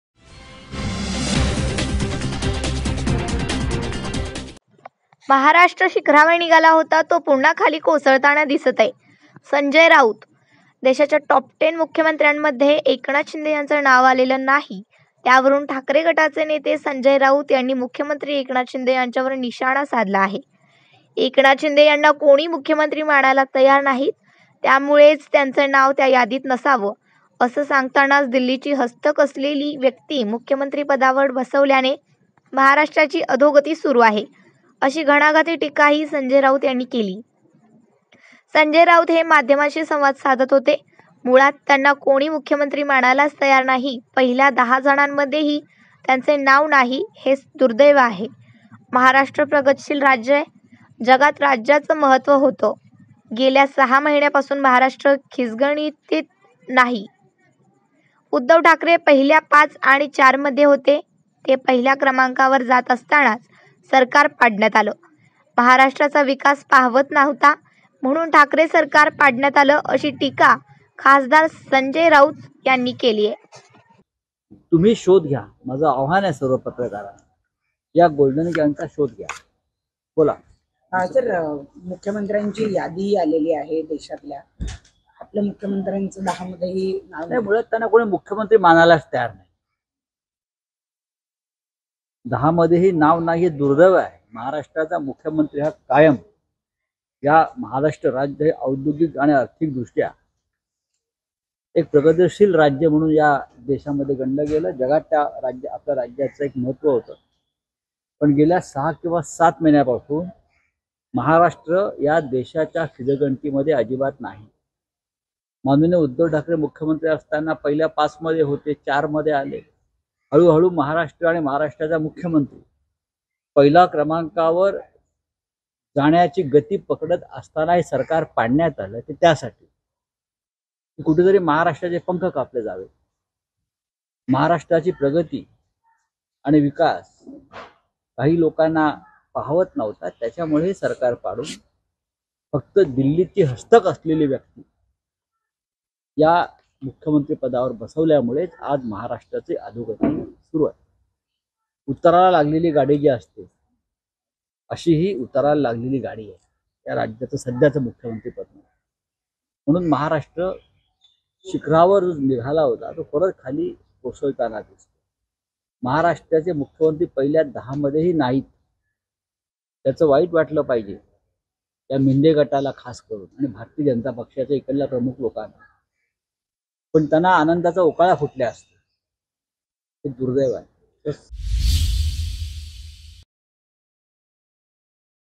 SANJAY RAUT | महाराष्ट्र शिखरावर निघाला होता तो पुन्हा खाली कोसळताना दिसत आहे : संजय राऊत
महाराष्ट्र शिखरावर निघाला होता तो पुन्हा खाली कोसळताना दिसत आहे : संजय राऊत